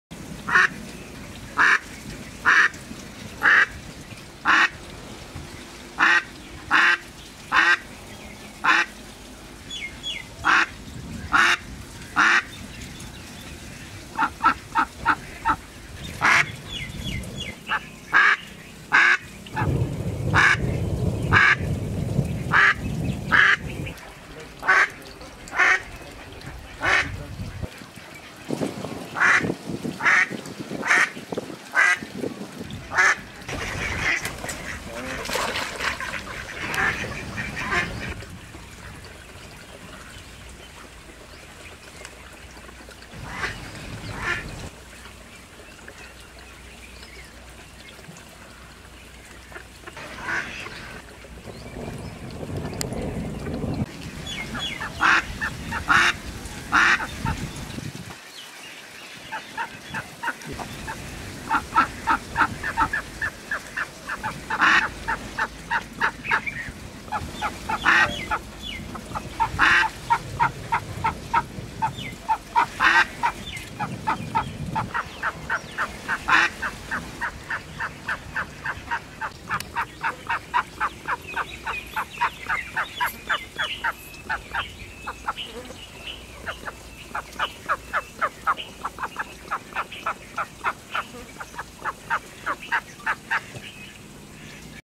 mallard duck sounds compilation || Mallard duck quacking call sounds || Learn Animal Sounds
The mallard or wild duck is a dabbling duck that breeds throughout the temperate and subtropical Americas, Eurasia, and North Africa, and has been introduced to New Zealand, Australia, Peru, Brazil, Uruguay, Argentina, Chile, Colombia, the Falkland Islands, and South Africa.